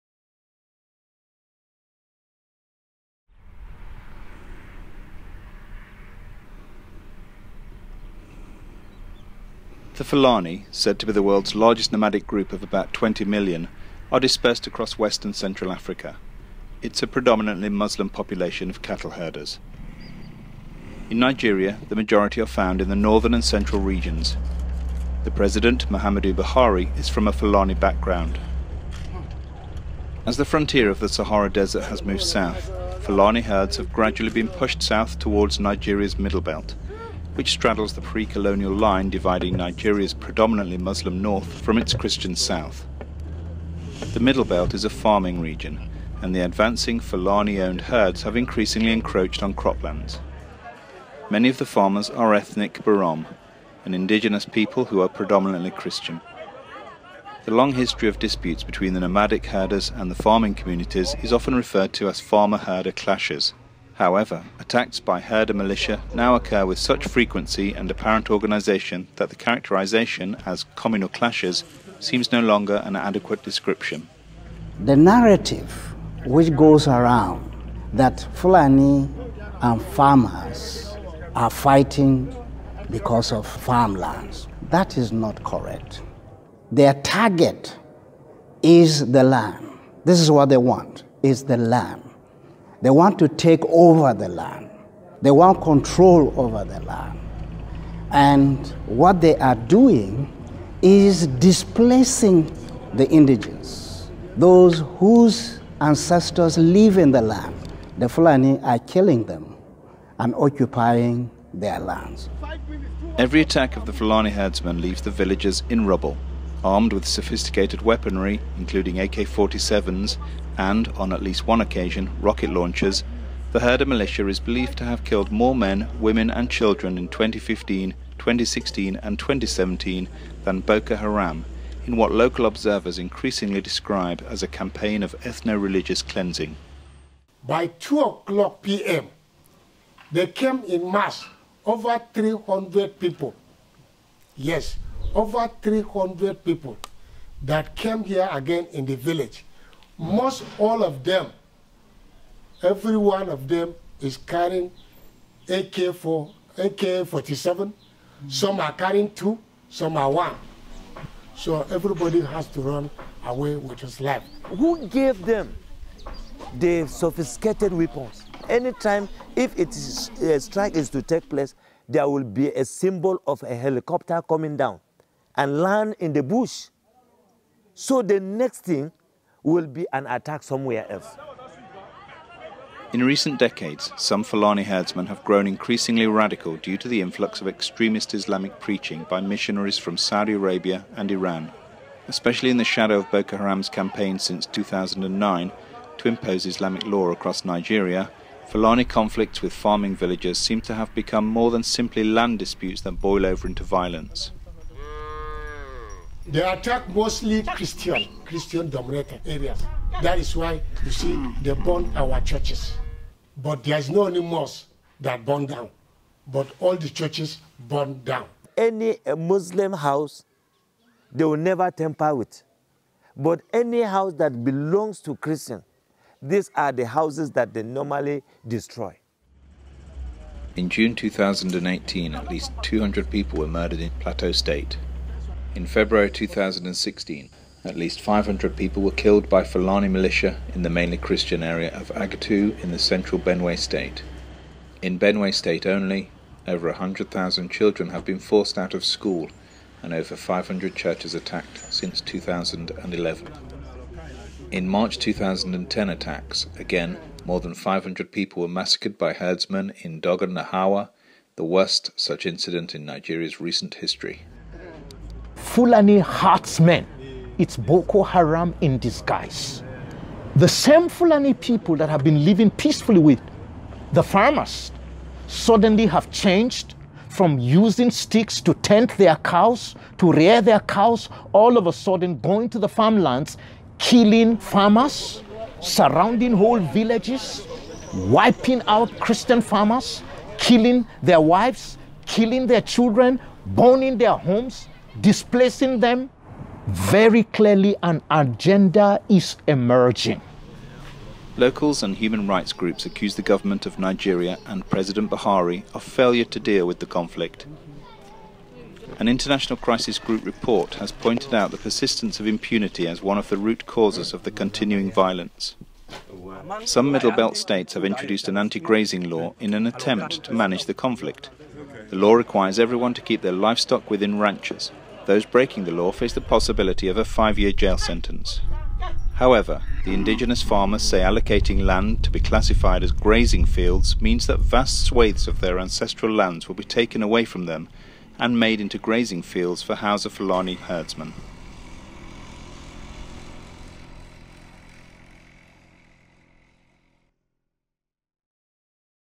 What is behind the Fulani herdsmen-farmers conflict?
World Watch Monitor explains the history of the Fulani herdsmen-farmers conflict and why, in recent years, the herders, who once lived peacefully alongside their Christian neighbours, have been responsible for more bloodshed than Boko Haram.